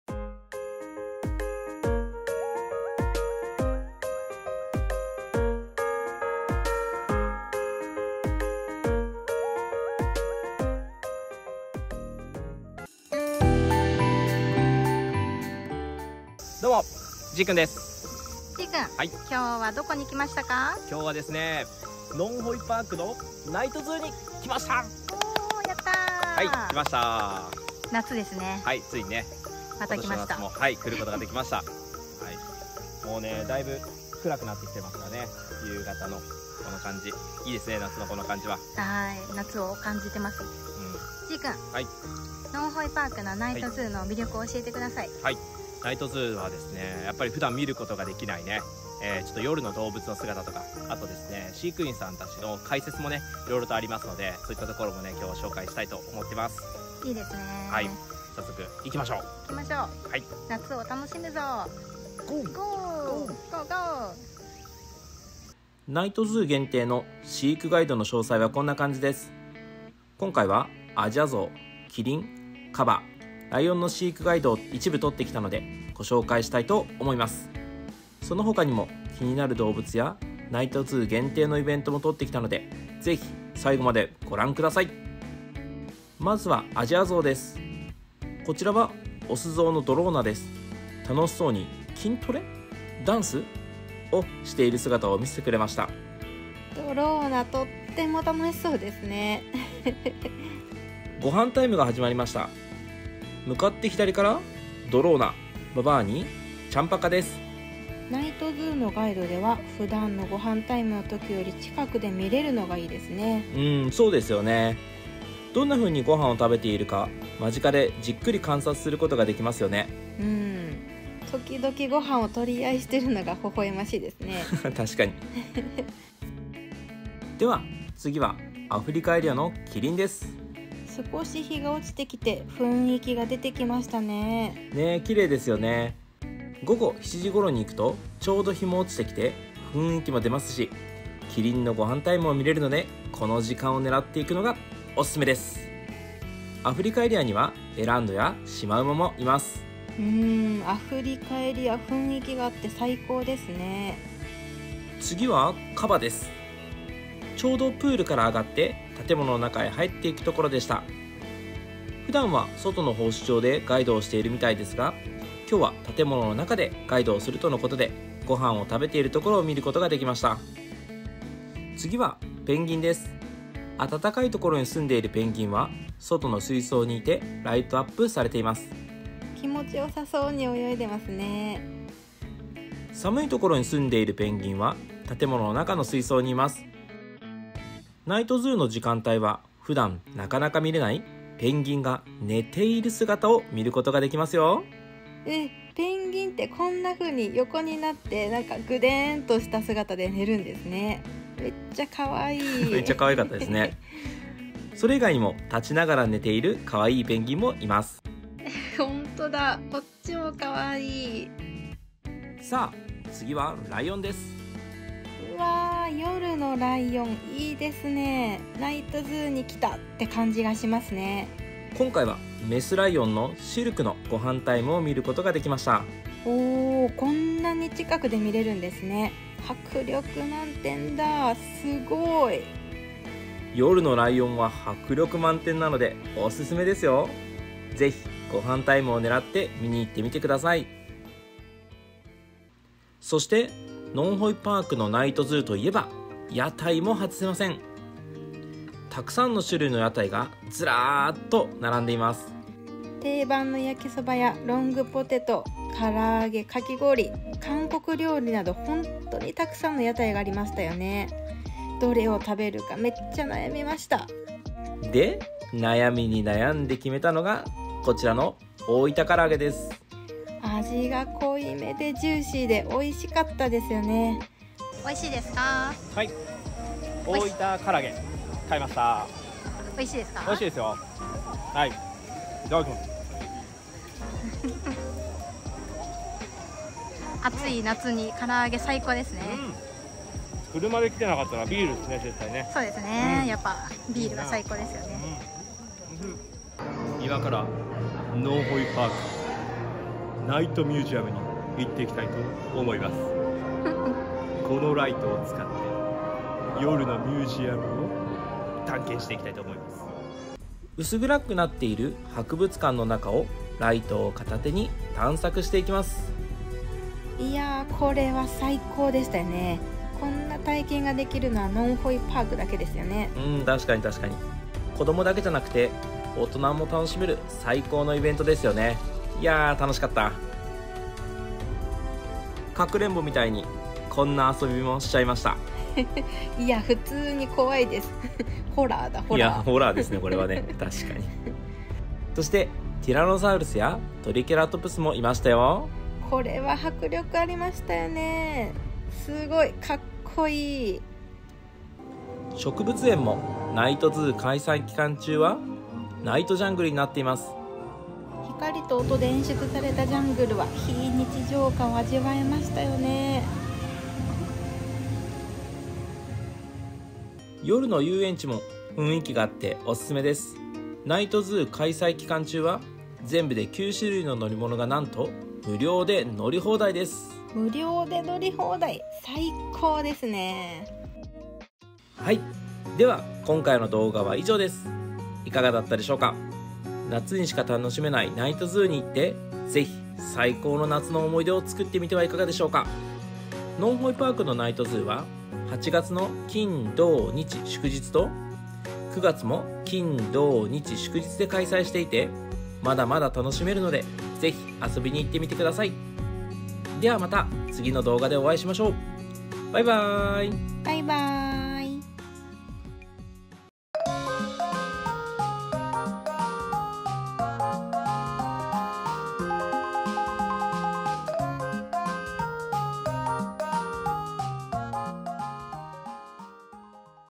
今年の夏も開催‼︎のんほいパークのナイトZOOが凄すぎる‼︎ お祭りだーー‼︎ドキドキ×ワクワク最大級
🔴のんほいパーク  ナイトZOO特設サイトはこちらから

🟡最新のオススメ動画はこちらから

🟠InstagramやTikTokの
YouTubeには載せていない動物達の可愛い動画も
沢山あります！